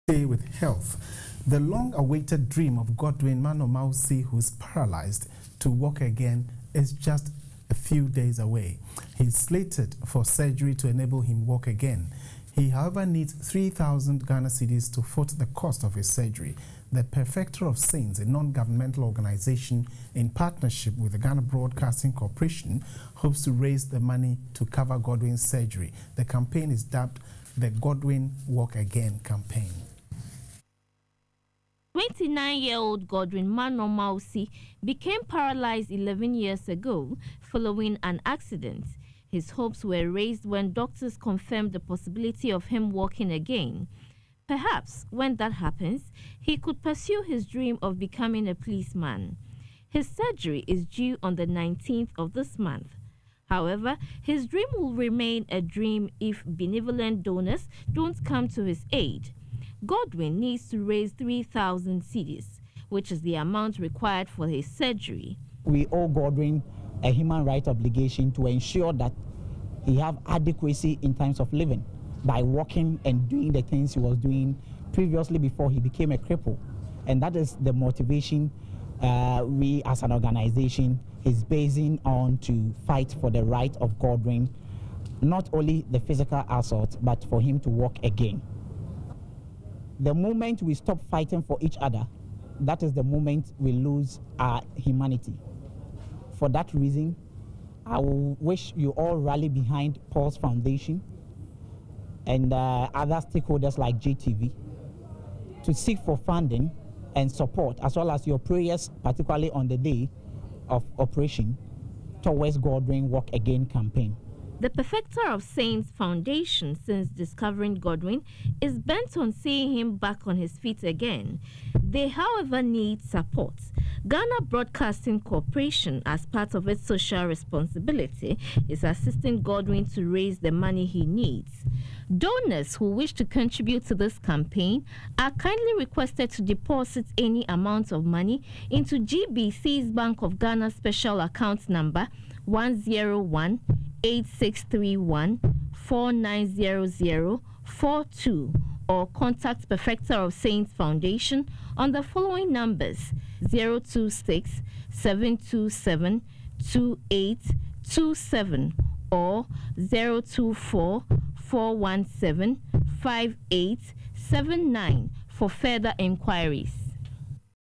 News on the Assaulted Cripple
POS Foundation goes to court 80 times over the assaulted case involving a 29 year old Godwin Manor Mawusi at Akim Swedru in the Eastern Region. Godwin became crippled as a result of been struck at the waist and leg with a cement block by his father and elder brother after they failed to use him in a spiritual ritual for money, therefore hated him.